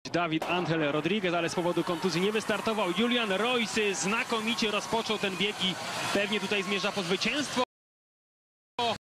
European Athletics Indoor Chamionshpis, Göteborg 2013, 60 m Men Rd 1 heat 1
1  CÎMPEANU, Catalin  ROU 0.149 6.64 Q
2  REUS, Julian  GER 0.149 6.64 Q
3  AIKINES-ARYEETEY, Harry  GBR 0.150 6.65 Q  SB
4  BIRON, Emmanuel  FRA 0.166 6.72 Q
5  FONGUÉ, Rolf  SUI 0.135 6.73
6  KLING-BAPTISTE, Tom  SWE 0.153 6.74    SB
7  ANTUNES, Diogo  POR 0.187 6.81
8  DE SA, Mikel  AND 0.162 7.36